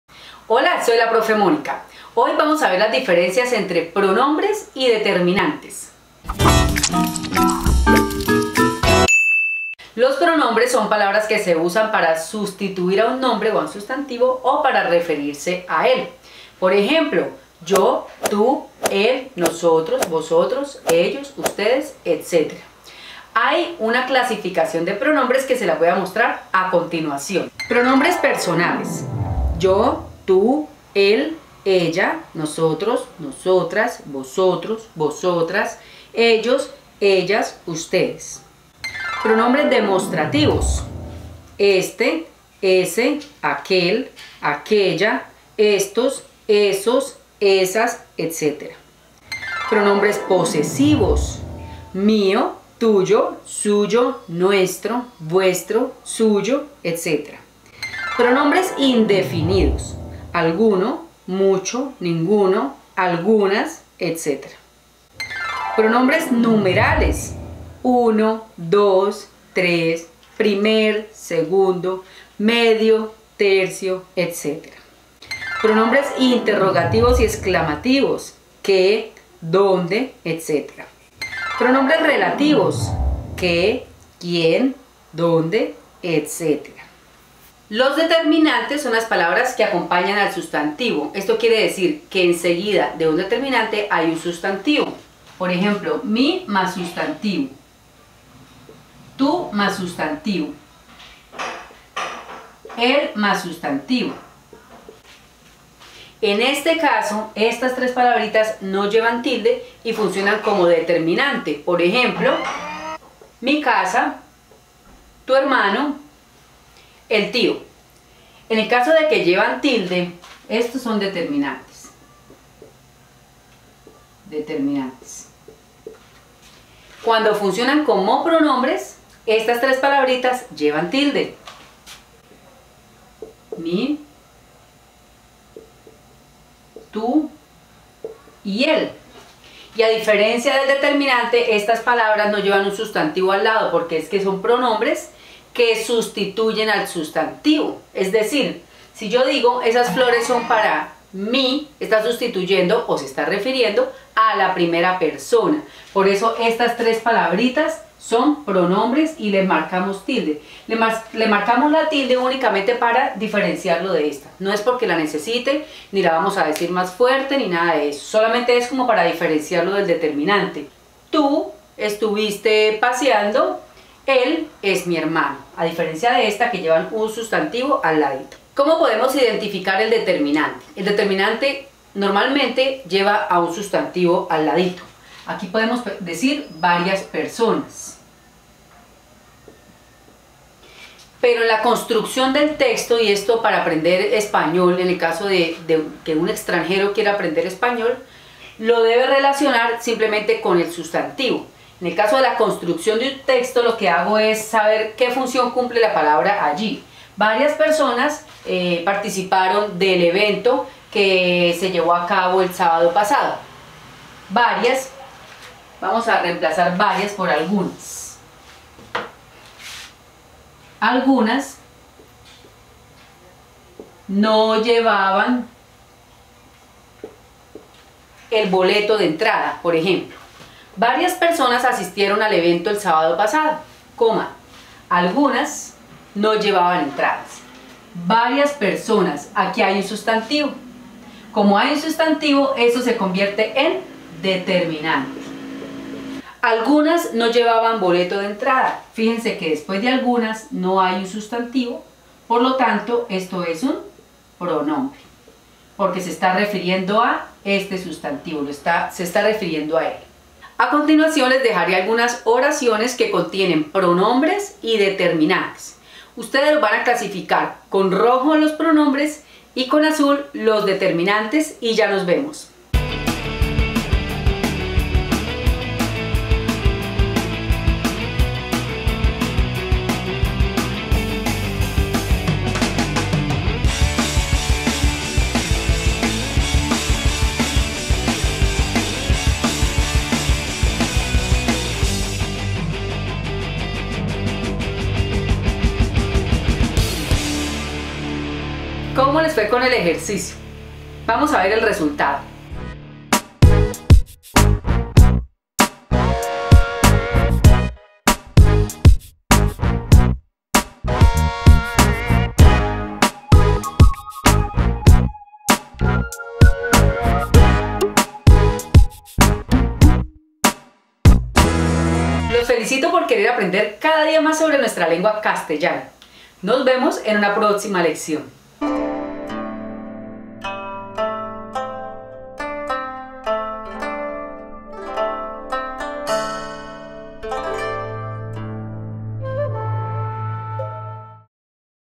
CÓMO DIFERENCIAR PRONOMBRES Y DETERMINANTES - gramática - edutuber
¡Ey! ey! ¡ey! ¿Cómo hacemos para saber si la palabra "varios" o "él" o "algunos" es pronombre o determinante? ¡Aquí les traigo unos superdatos para que no se confundan más! ¡Bienvenidos! ❤️

Recuerda que en este canal encuentras más videos de gramática y sintaxis.